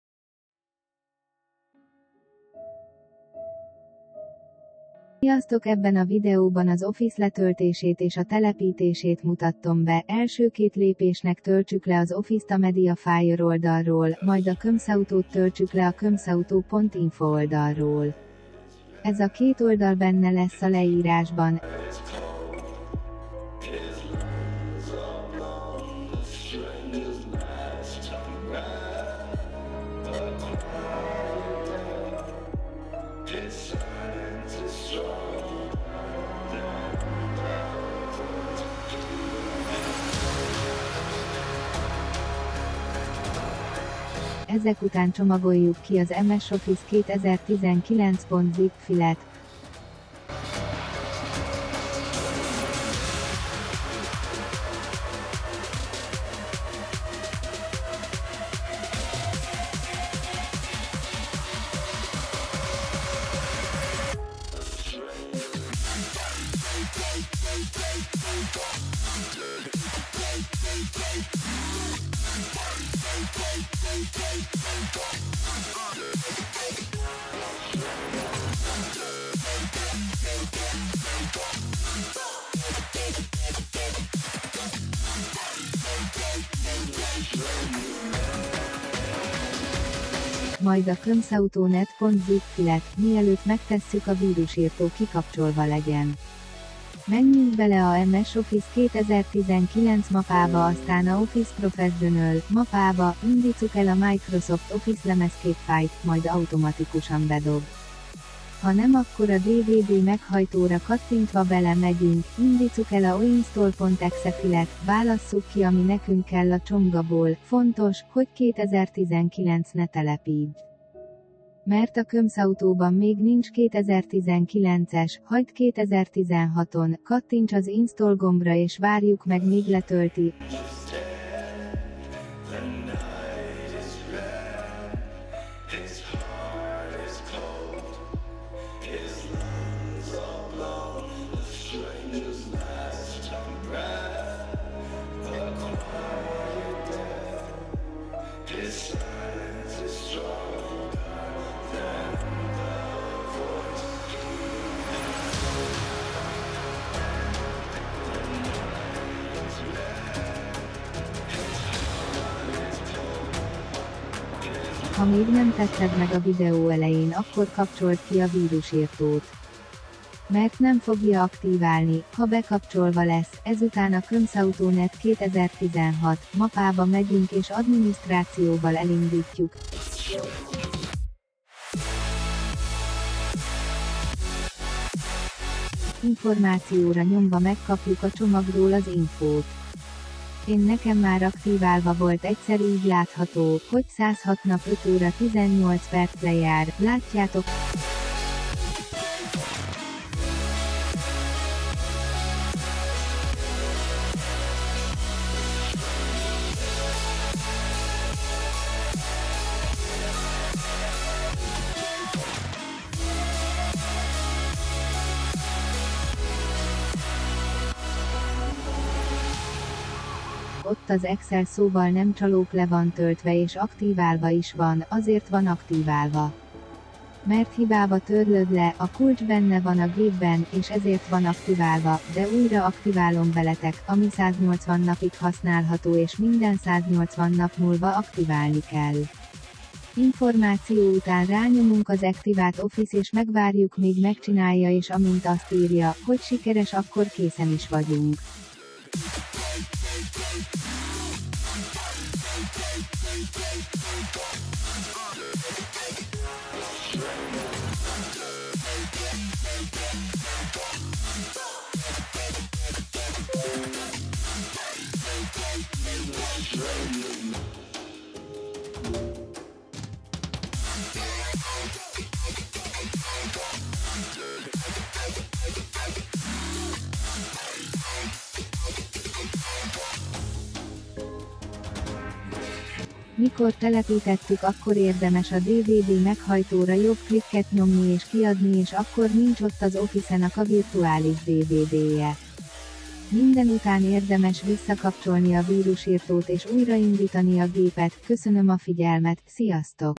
Office csomag letöltése és telepítése | Letöltések tutoriál | HU/EN
Sziasztok!
A mai videóban az office csomag letöltését és telepítését mutatom be.